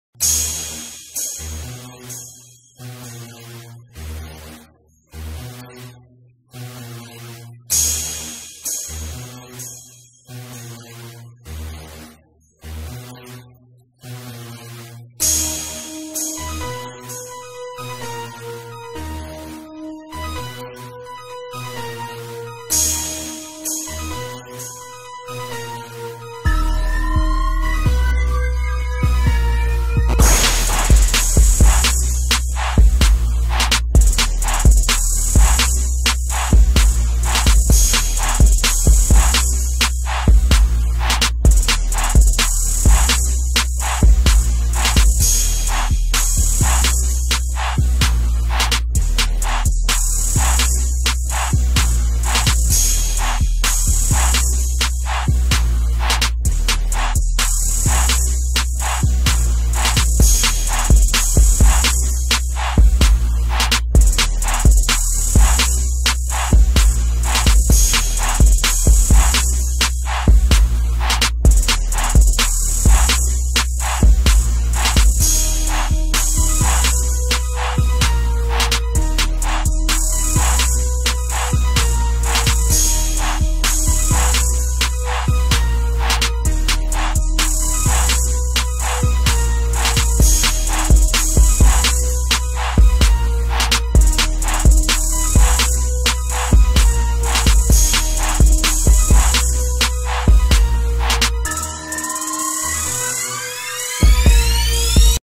YOUNG CHOP DRUM SOUND KIT Trap Samples Download
YOUNG CHOP DRUM SOUND KIT
WITH THIS AMAZING YOUNG CHOP DRUM SOUND KIT YOU WILL GET ALL SAMPLES AND SOUNDS TO PRODUCE BEATS IN THE STYLE OF CHICAGOS TRAP BEATMAKER YOUNG CHOP.
A TOTAL OF 149 TOP QUALITY WAVE SAMPLES AND 15 SOUNDFONTS (PIANOS,BELL,SYNTHS,BRASS,STRINGS AND MORE) NOTE: REMEMBER ITS ABOUT QUALITY NOT QUANTITY OF THE SOUNDS, FORGET THOSE FAKE DRUM KITS FILLED WITH GBs OF USELESS GARBAGE...